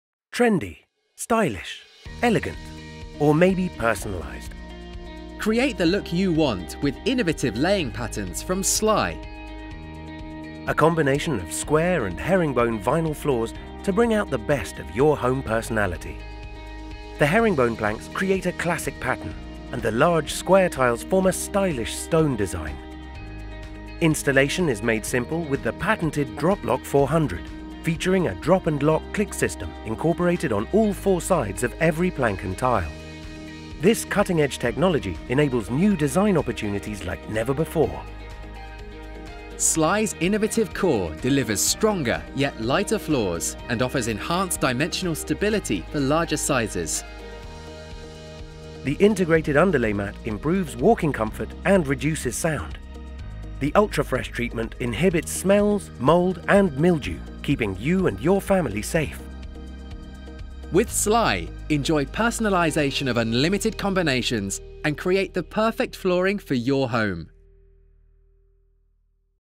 Combined SLY Square & Herringbone Design Floors
SLY SQUARE & SLY HERRINGBONE

The inovative Droplock 400 system stands out, characterized by ”drop-and-lock” click connections on all four
sides of the panels. As a result, different wood patterns
can be put together with this product. An enormous
variety of installations are available, which make your
space an eye-catcher. With click connections on all four
sides, there are no limits to your imagination. Countless
combination possibilities let your rooms shine.